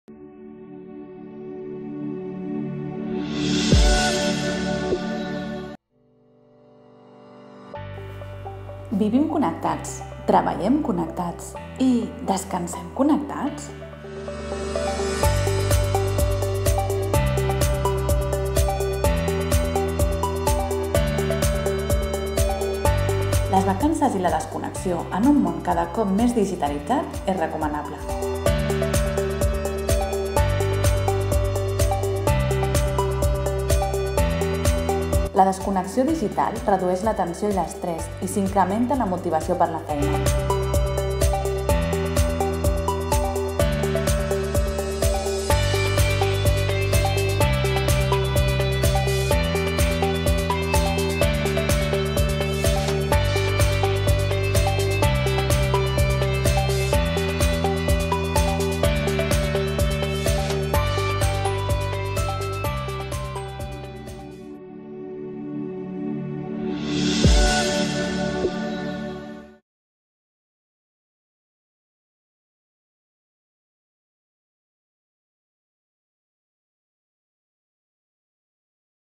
Desconnexió digital saludable
L'equip de BSA APPediatria, l'aplicació mòbil del Servei de Pediatria, ha preparat aquest vídeo convidant a una desconnexió digital saludable.